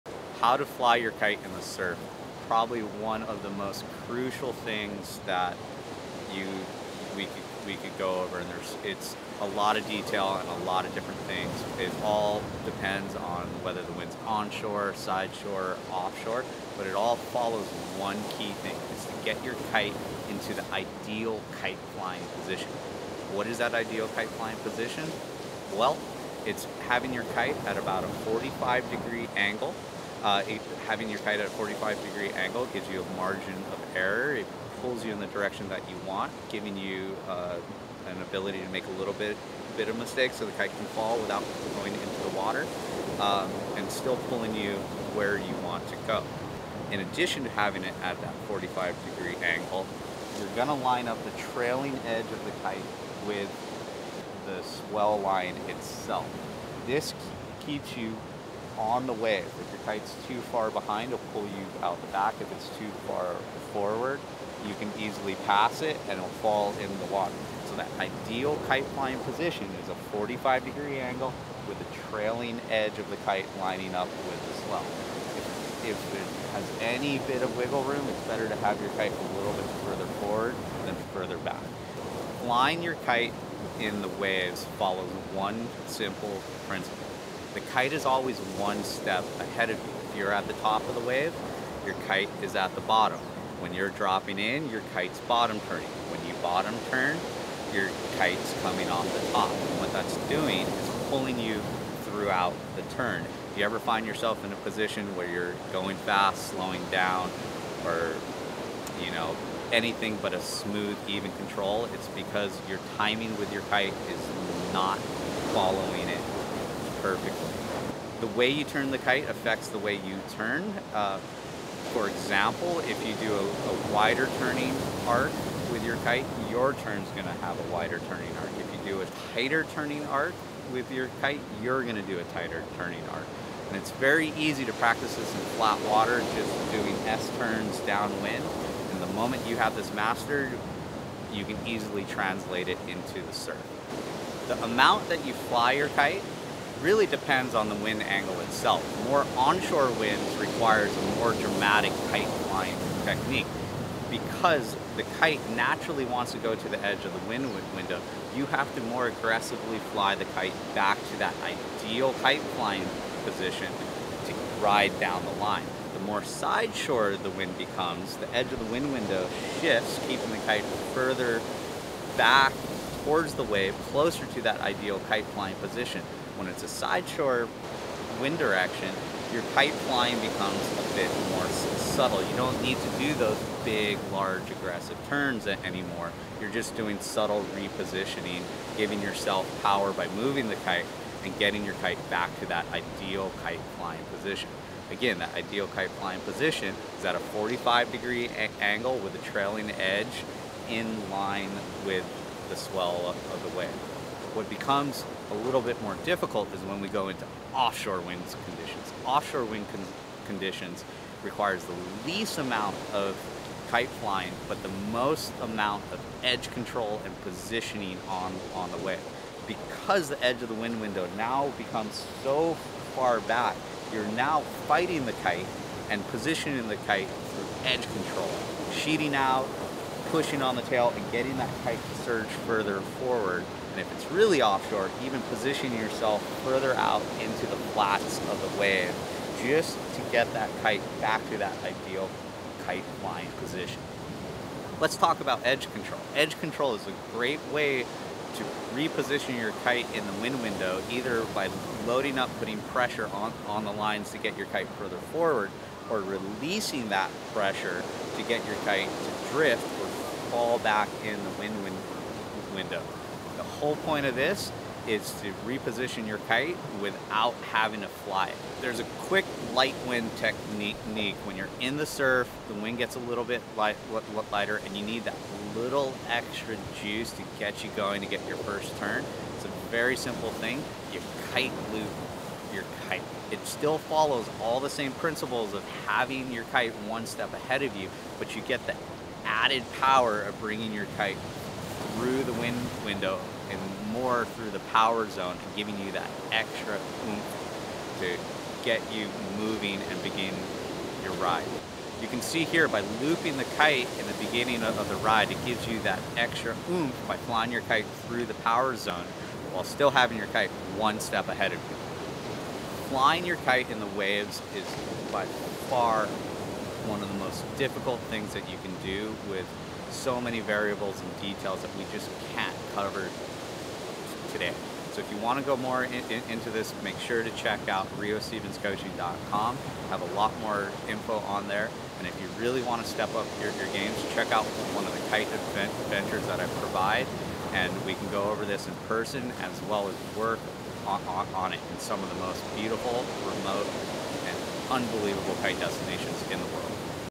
How to Fly Your Kite in the Surf - How to Kitesurf
Basic concepts of how to fly your kite in the surf while kitesurfing in a variety of wind directions.